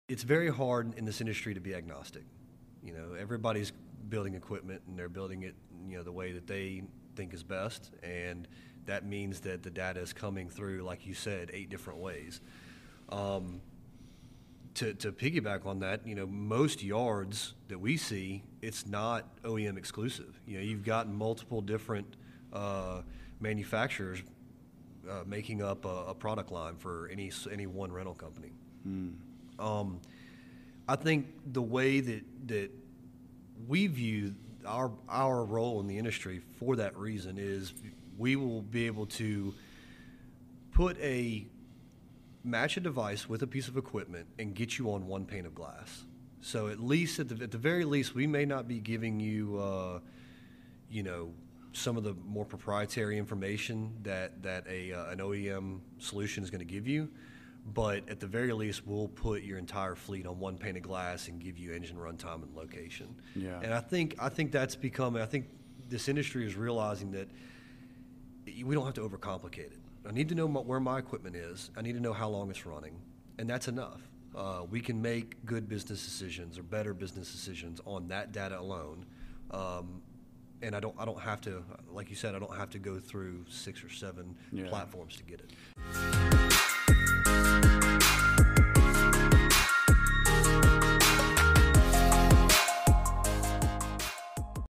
Justin Walters - says they will gather all fleets together and provide engine runtime and location.
Justin Walters of Geoforce admits that they won't be able to share more proprietary data, such as OEM solutions do, but they will gather all fleets together and provide engine runtime and location.